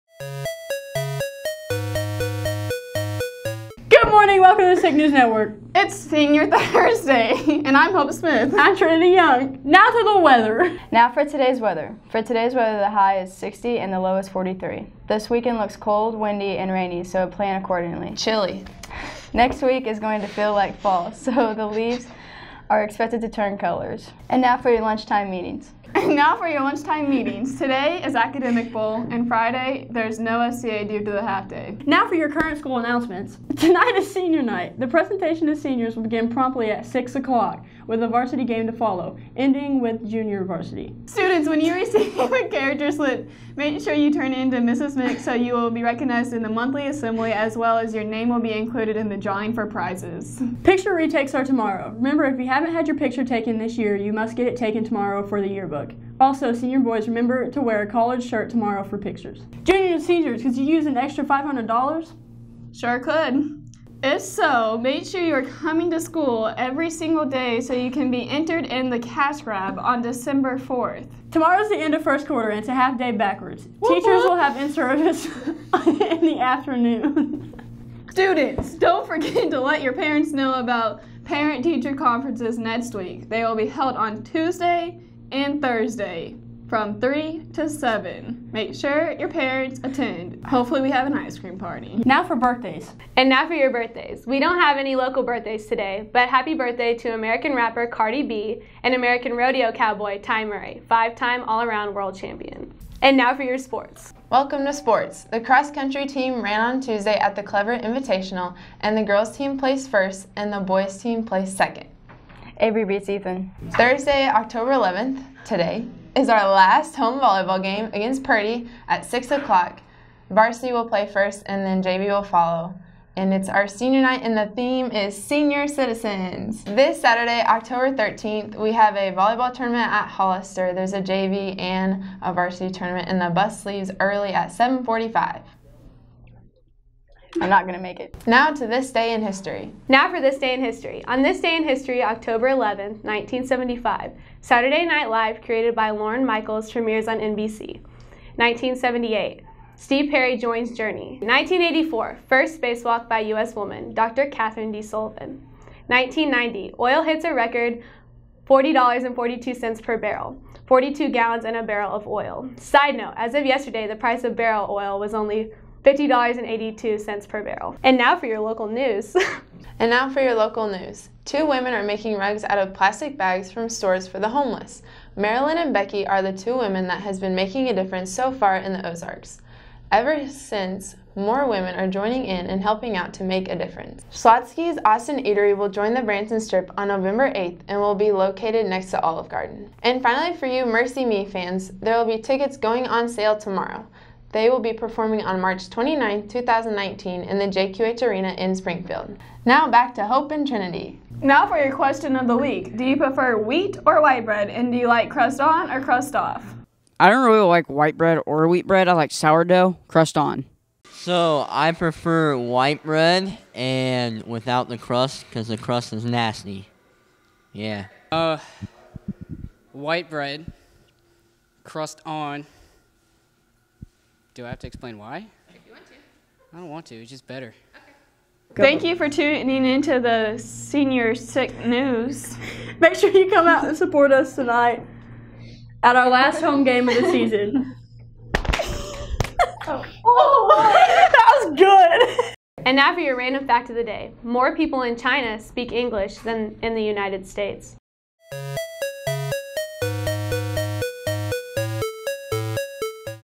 Senior Special Edition, Sick Newz Thursday, October 11
Special Senior Edition of the Sick Newz. The 5 seniors on the volleyball team takeover the news today.